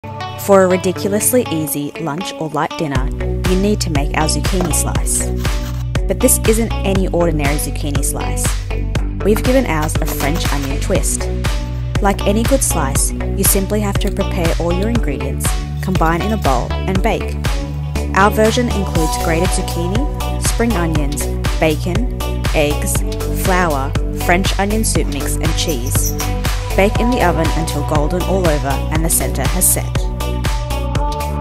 French onion zucchini slice with bacon
Two favourites combine here to create a flavour-packed slice that can be enjoyed any time of the day. There’s no need to season the slice with salt as the French onion soup mix, cheese and bacon do that for you.